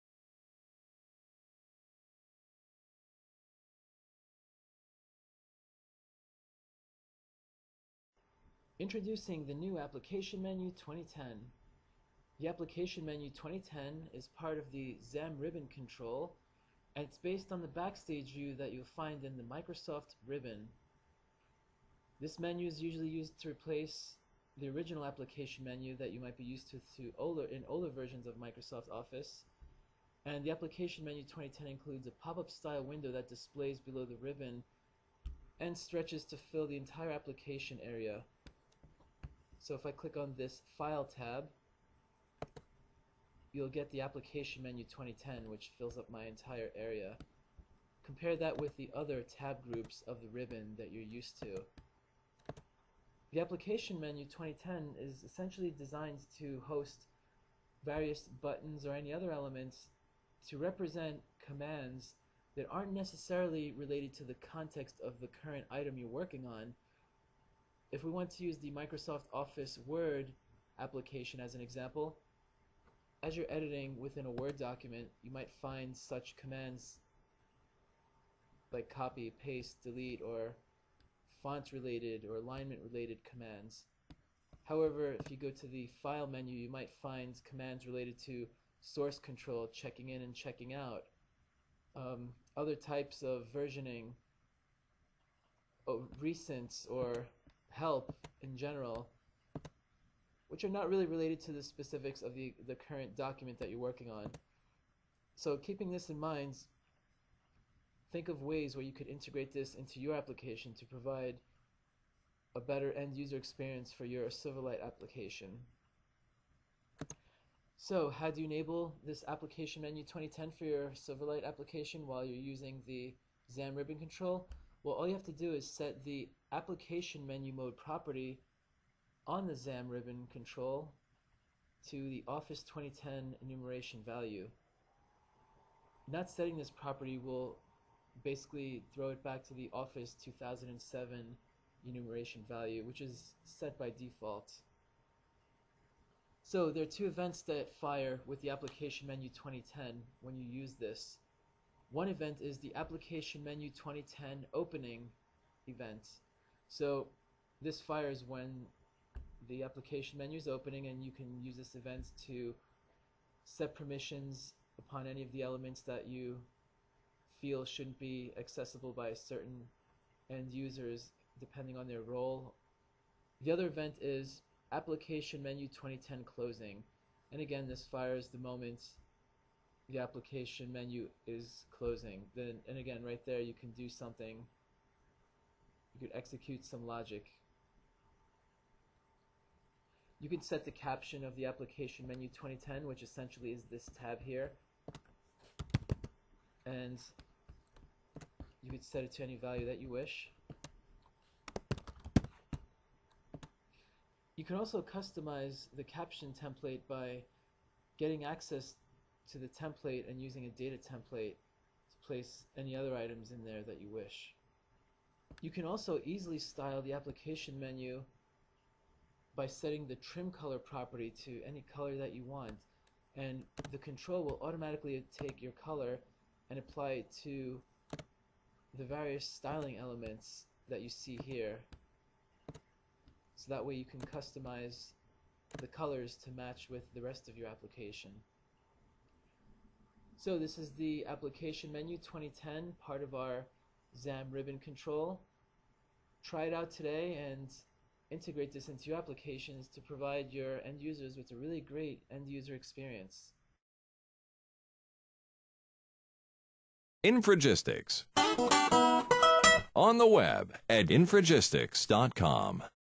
xamRibbon Application Menu 2010
Introduction to the xamRibbon control's Application Menu 2010 view style.